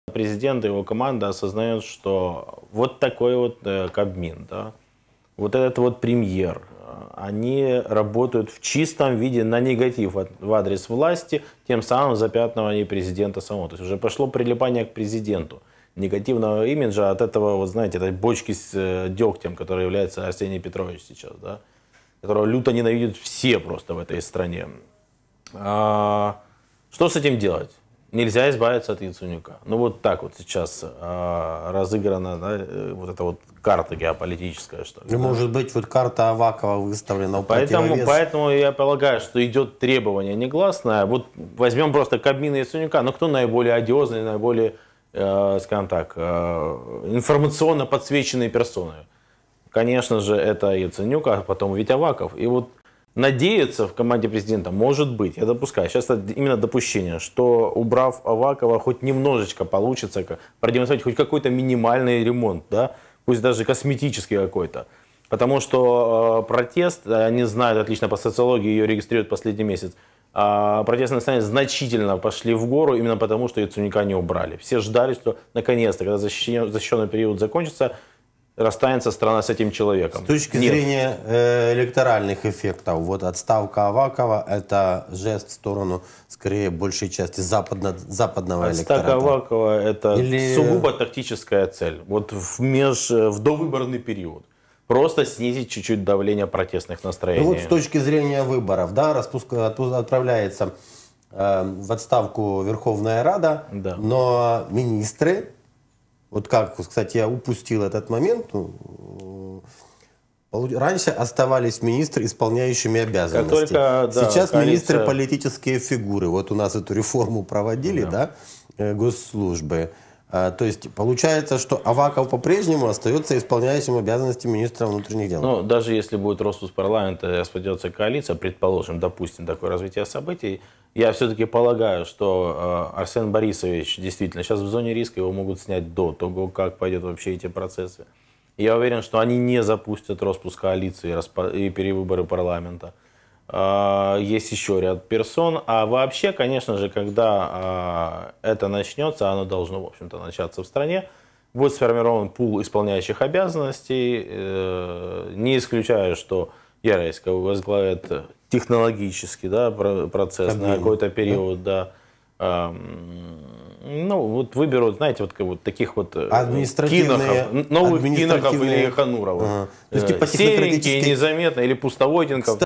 Яценюк стал "бочкой дегтя" для Порошенко
Чтобы как-то нивелировать общественное недовольство действующим премьером, команда президента потребует от Арсения Яценюка замены главы МВД Арсена Авакова на любую другую кандидатуру. Об этом в эфире 17 канала заявил политический эксперт Михаил Павлив.